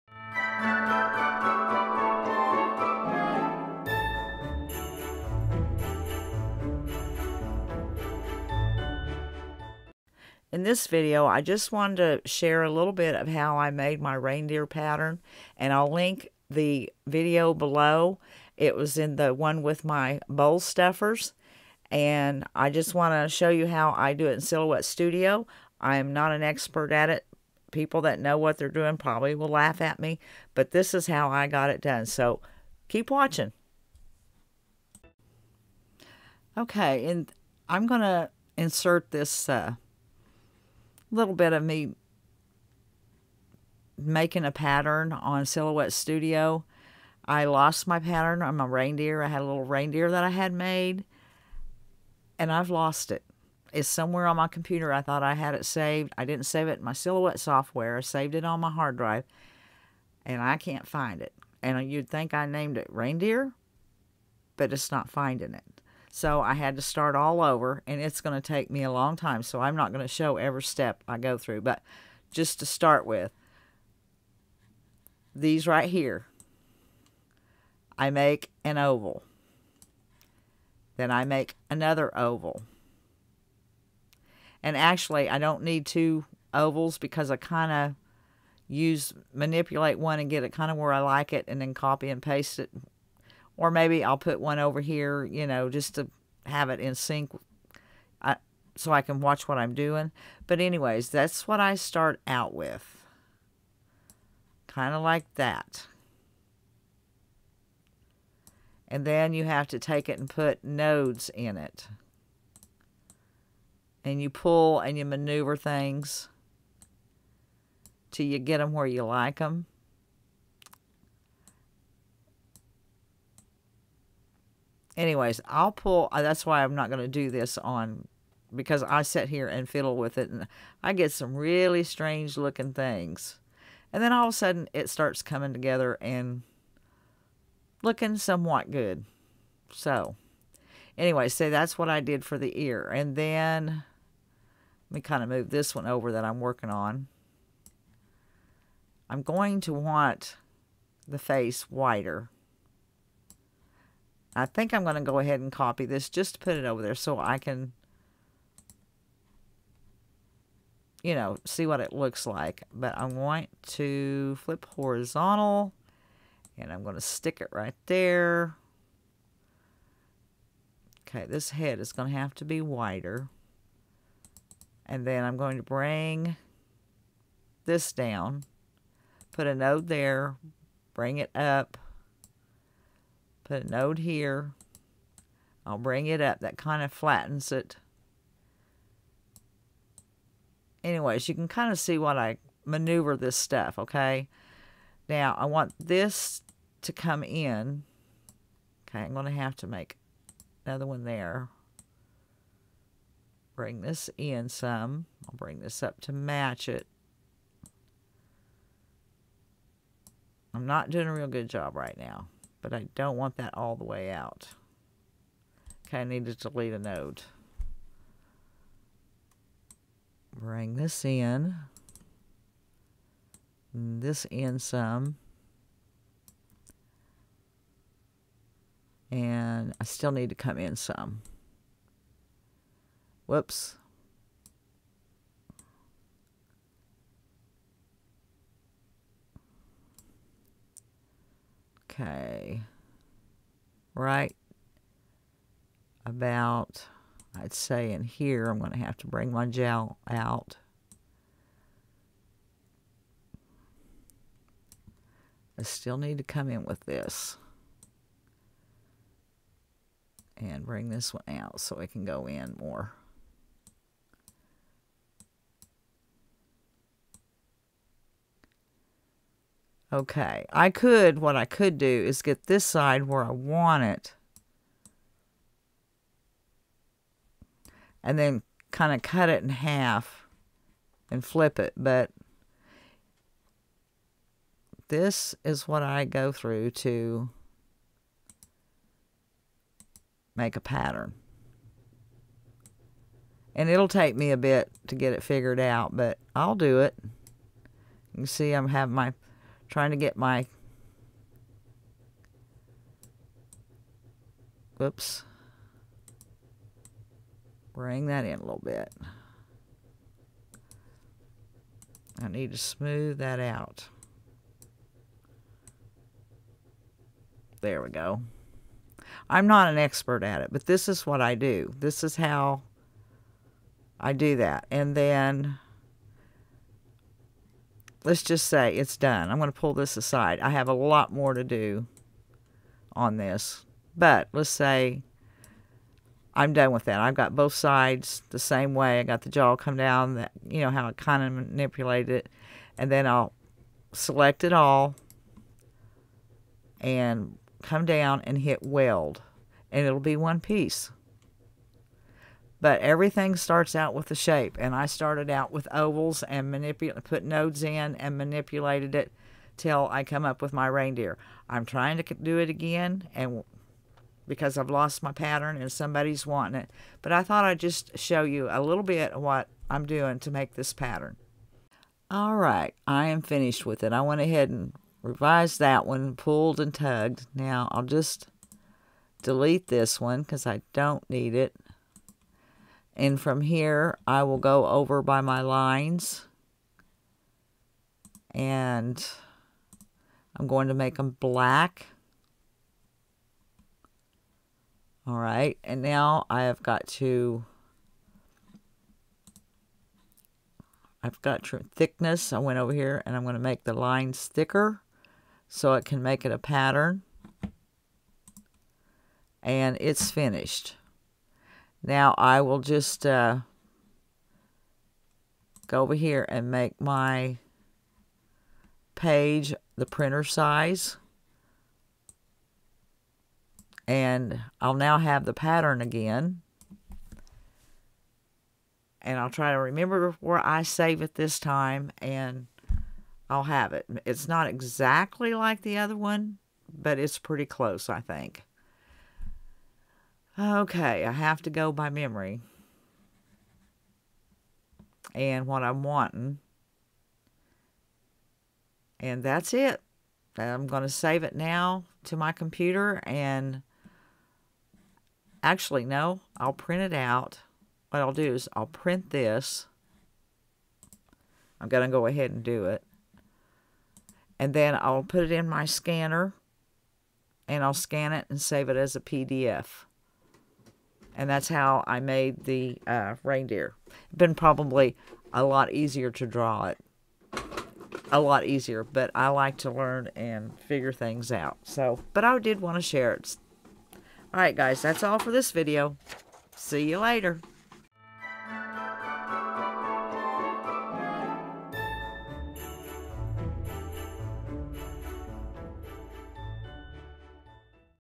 Making Reindeer Patten In Silhouette Studio
I'm showing how I design a pattern in Silhouette Studio.  It all starts with a shape.  I am no where a pro at doing this but like to learn.  We all have to start somewhere!